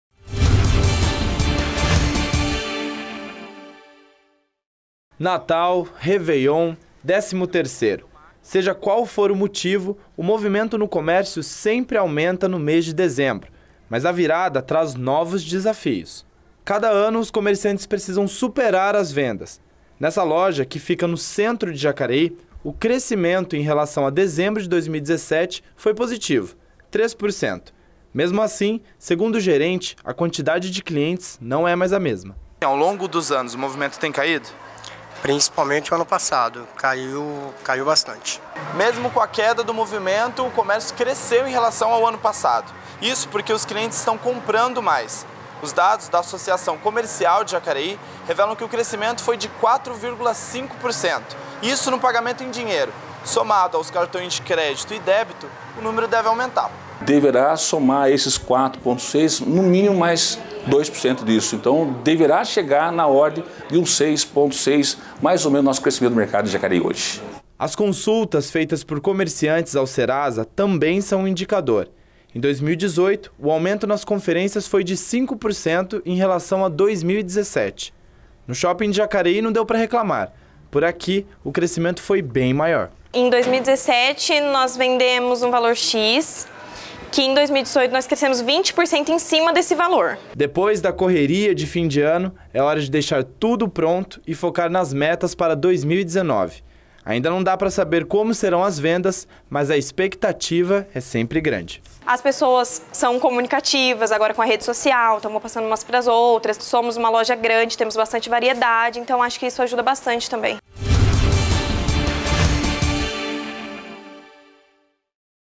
Banco do Comércio 2018/2019
Seja qual for o motivo, o movimento no comércio sempre aumenta no mês de dezembro, mas com a virada de ano, novos desafios surgem. Confira no vídeo.